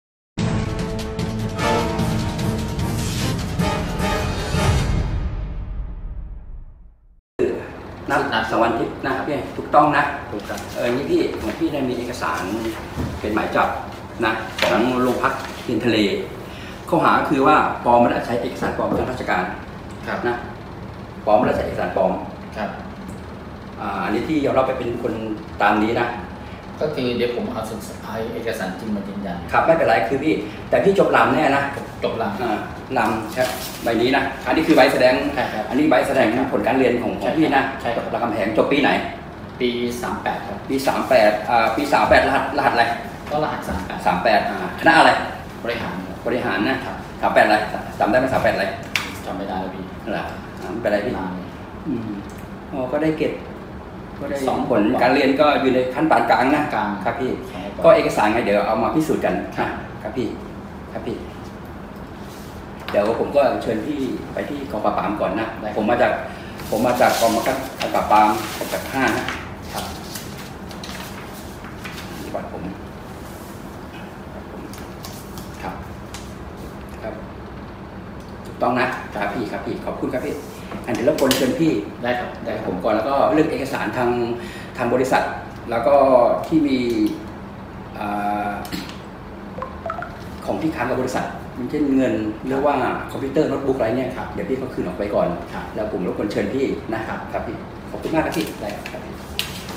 ตระเวนใช้วุฒิปลอม ม.รามฯ สมัครงานบริษัท จนถูกไล่ออก
15/11/2018 ตระเวนใช้วุฒิปลอม ม.รามฯ สมัครงานบริษัท จนถูกไล่ออก
MGR online -กองปราบรวบหนุ่มใหญ่สุดแสบตระเวนใช้วุฒิปริญญาตรีปลอม ม.รามฯ สมัครงานบริษัท จนถูกตรวจสอบ ผู้ต้องหาให้การสารภาพซื้อมา 7 พันบาท